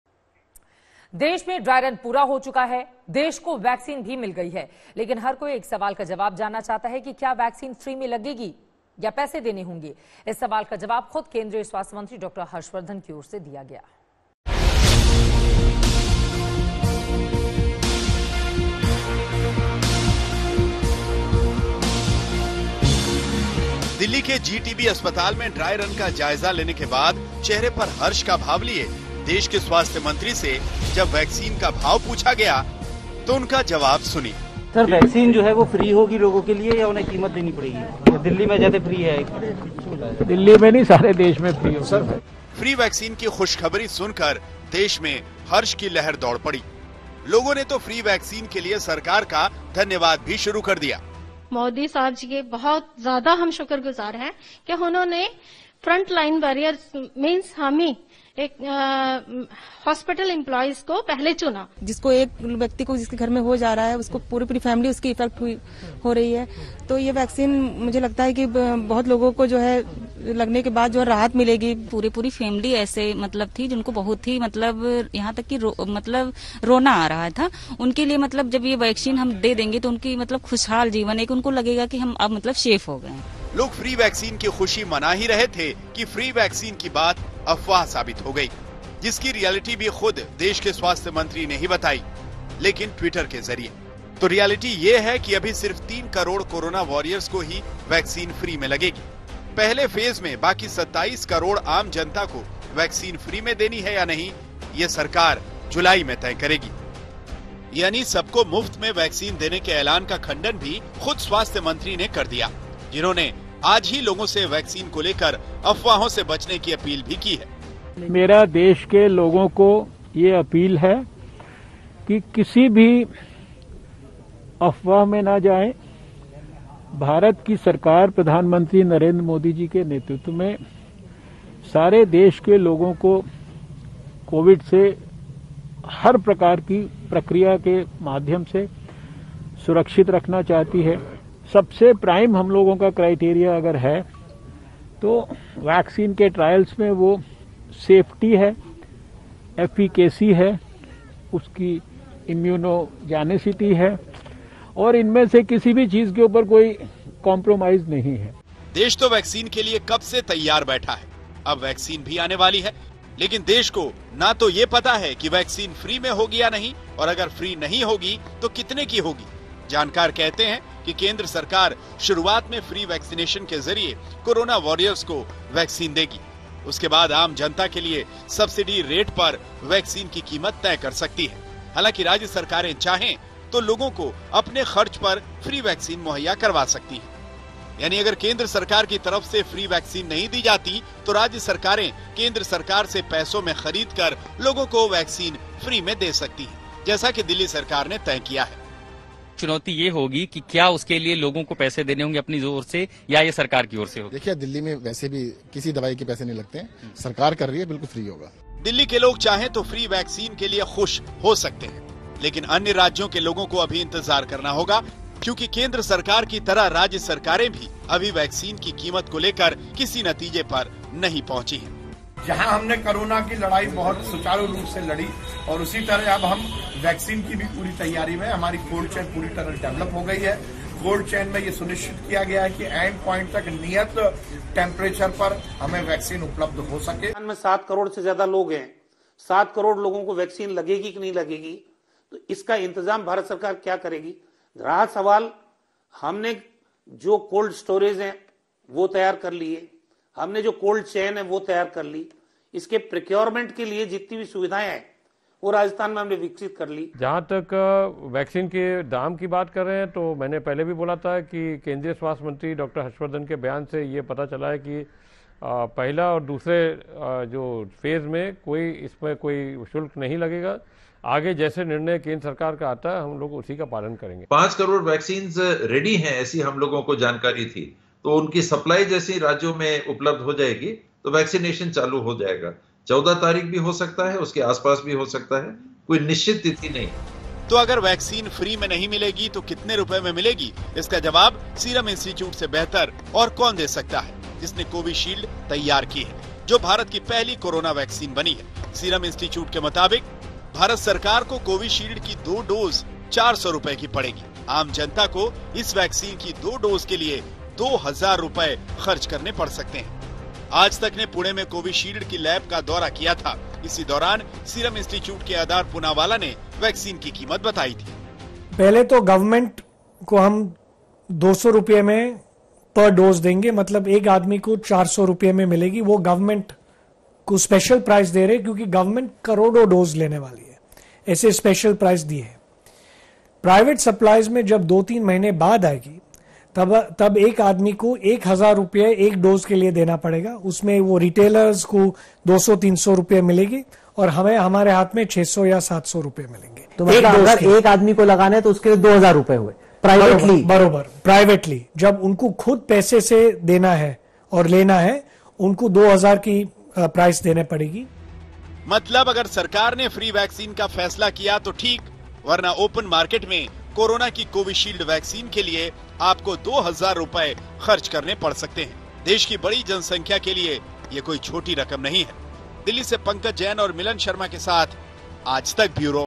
Corona Vaccine: Free में लगेगी या फिर देने होंगे पैसे, सरकार का प्लान क्या?
देश में वैक्सीन का ड्राई रन पूरा हो चुका है. देश को वैक्सीन भी मिल गई है. लेकिन हर कोई एक सवाल का जवाब जानना चाहता है. क्या वैक्सीन फ्री में लगेगी? या पैसे देने होंगे? इस सवाल का जवाब खुद केंद्रीय मंत्री हर्ष वर्धन की ओर से दिया गया.